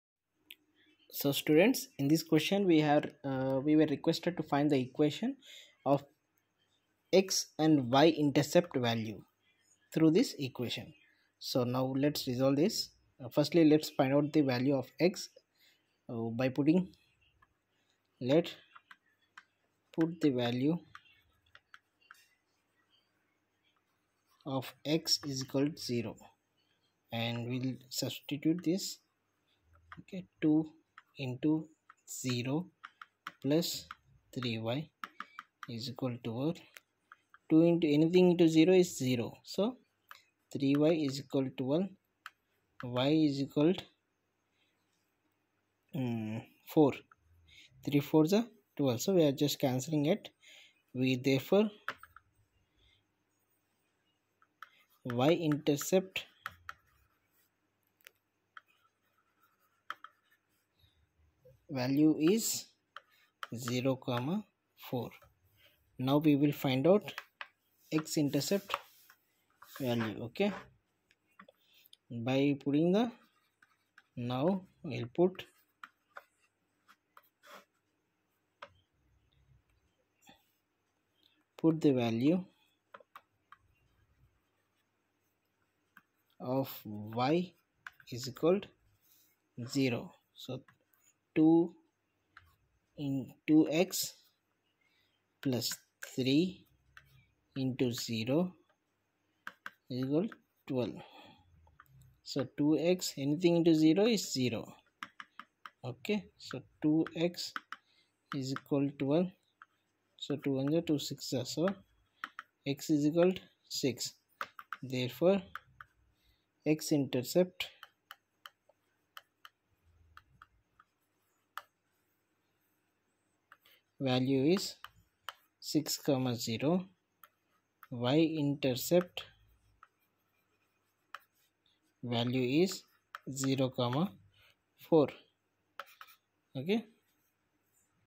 Find the x and y intercepts from the equations 2X + 3Y = 12
Find the x-and y-intercepts of the following equations.

2x + 3y = 12

Solution:

2x + 3y = 12

Put the value of x = 0

2 × 0 + 3y = 12

3y = 12

3y/3 = 12/3

y = 4

Therefore, y-intercept = (0, 4)

Now, put the value of y = 0

2x + 3 × 0 = 12

2x = 12

2x/2 = 12/2

x = 6

Therefore, x-intercept = (6, 0)

how to graph a linear equation